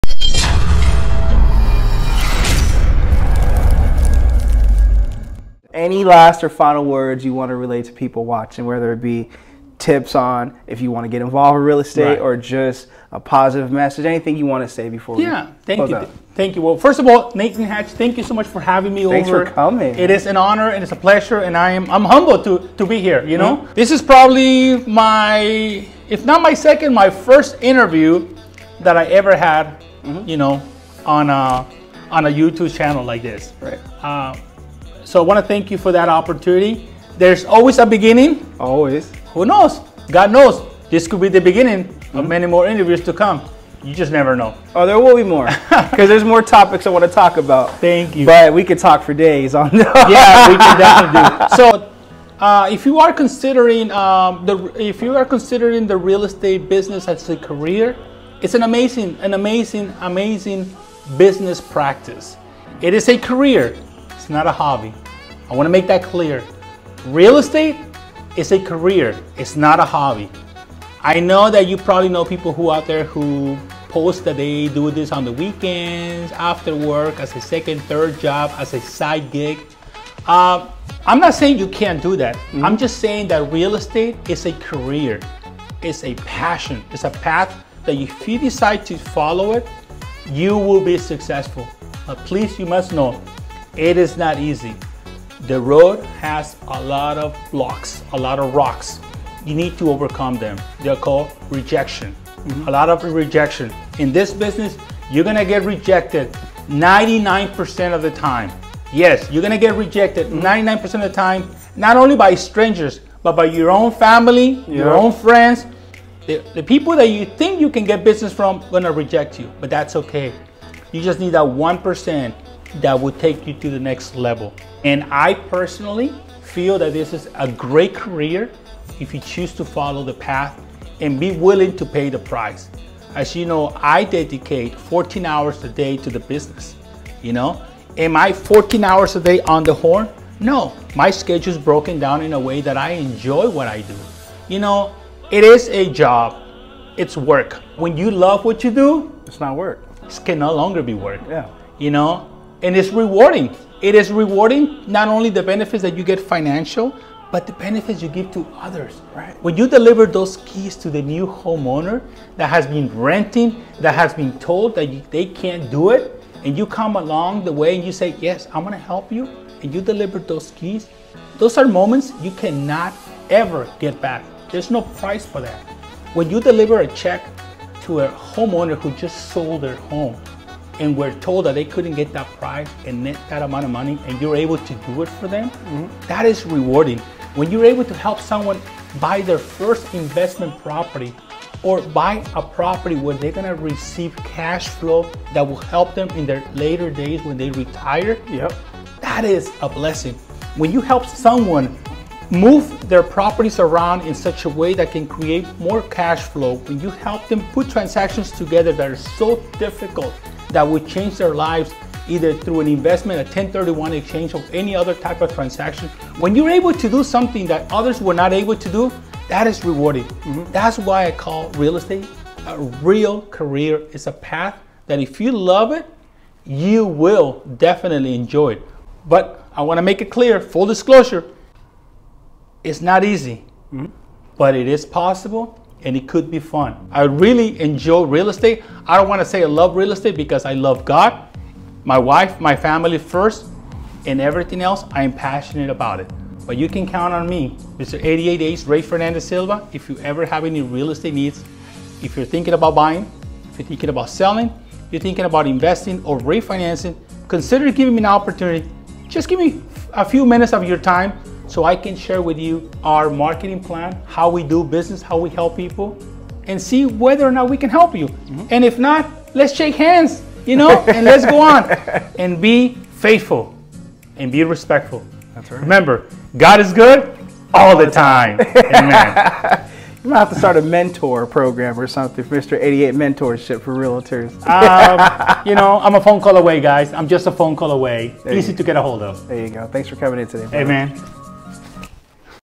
Real Estate Tips for Beginners [ New Agents ]
First I would like to thank our Lord and Savior. Without Him, none of this would be possible. Real Estate tips for beginners and new agents.

Ray Fernandez Silva aka Mr. 88 days gives his short but impactful input on Real Estate as a career.

This is a must watch! Hope you enjoy. Stay blessed.

My Books

📙Additional Ways to Earn More They Don’t Teach
📖How to Find You
📕The Come Up

Hope you enjoy. Stay blessed